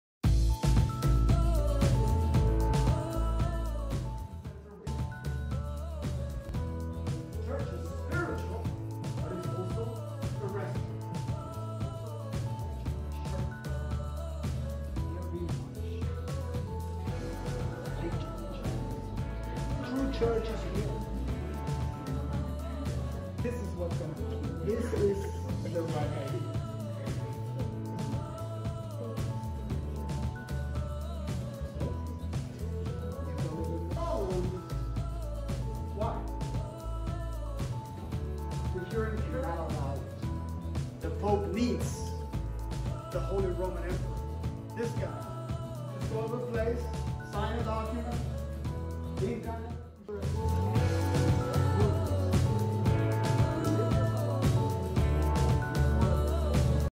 【授業紹介】ヨーロッパ：変革から革命への移行Europe: Reformation to Revolution（3・4年対象）
国際教養学部3・4年対象授業
「ヨーロッパ：変革から革命への移行　Europe: Reformation to Revolution」
の約1分紹介動画です！